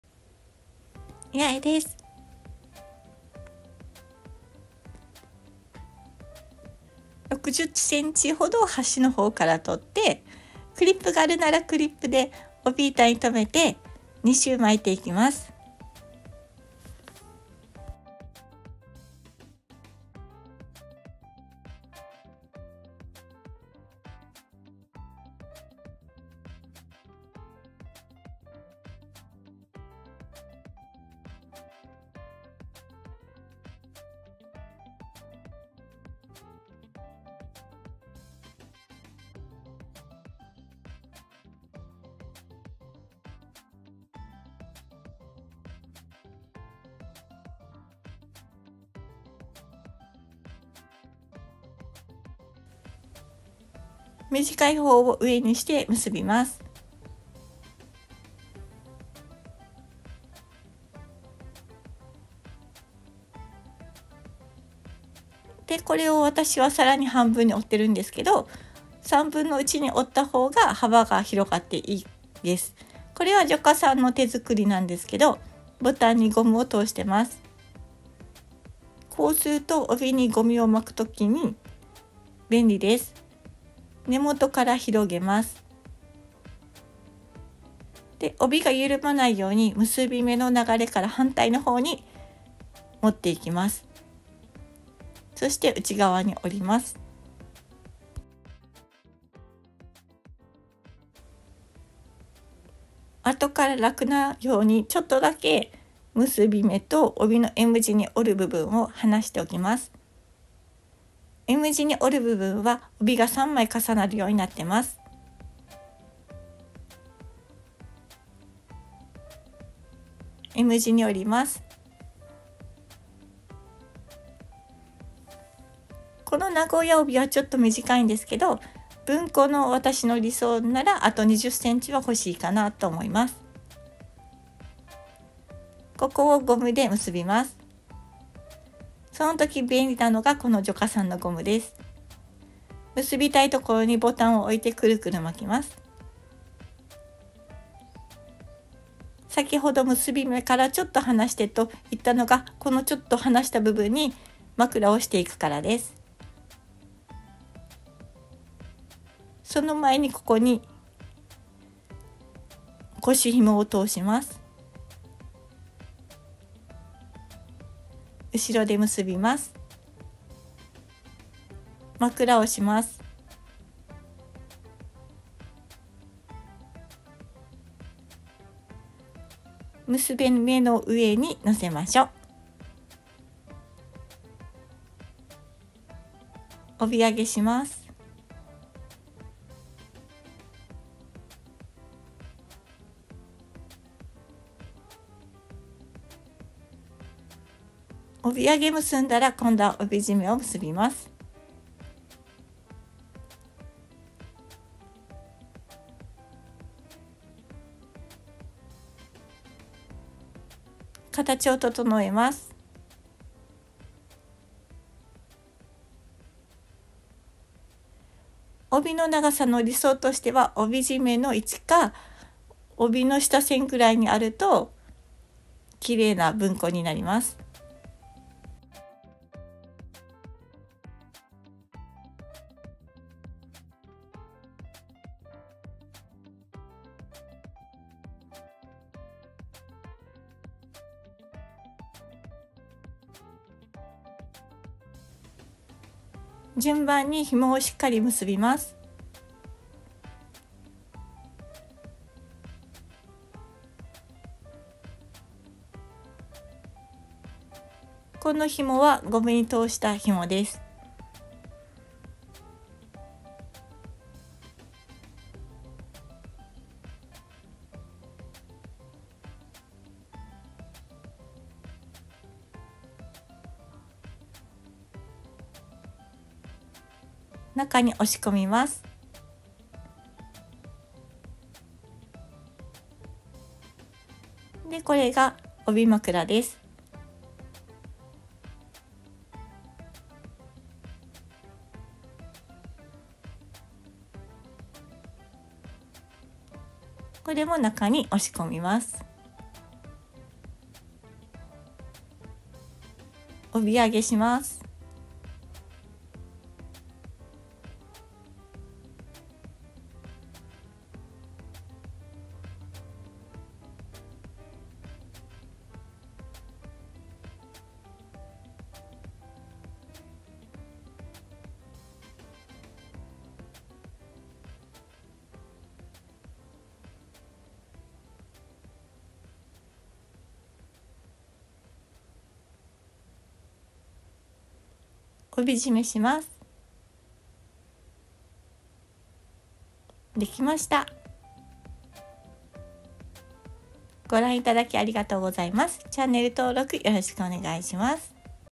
Bunkomusubi
Bunko tied with a nagoyaobi . It's a little short obi, so it feels like it's not long enough to tie, but I think it.

If you tie it with a Nagoya belt, it looks cool like a samurai woman of the Edo period
Please make sure to tie it tightly on New Year's Day

Please register for the channel

Home page
Joka

Yae

Joka

Yae